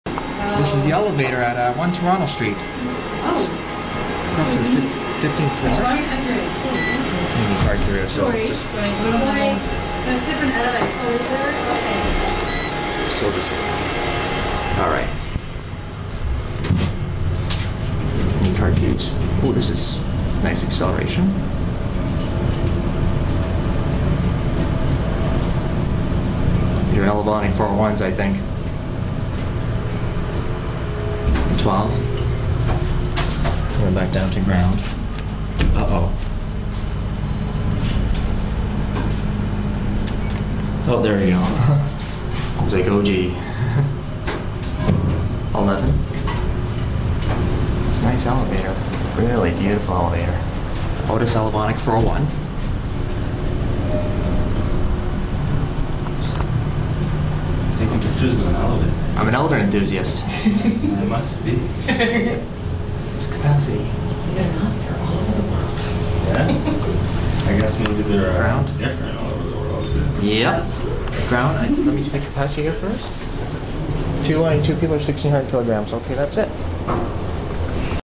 OTIS Elevonic 401 High Speed Elevators @ One Toronto Street (8-15)
Riding the very nice and fast elevators at One Toronto street in Toronto, ON. It's pretty interesting that a 15-story building has two elevator banks serving different floors. This building is also known as 70 King Street East.

Tech. Specs:

(Low-Rise Elevators 2-7)
General: OTIS/2000T Geared Traction
Capacity: 22 people/1600kg/3500lbs
Speed & Distance Traveled: 7 floors/75ft/400-500FPM
Power: 10HP
Door Safety: Sensor
Year Installed: 1989

(High-Rise Elevators 8-15) (Card Access)
General: OTIS/Elevonic 401 Gearless Traction
Capacity: (See Low-Rise)
Speed & Distance Traveled: 15 Floors/160ft/700-800FPM
Power: 30HP
Door Safety: Sensor
Year Installed: (See Low-Rise)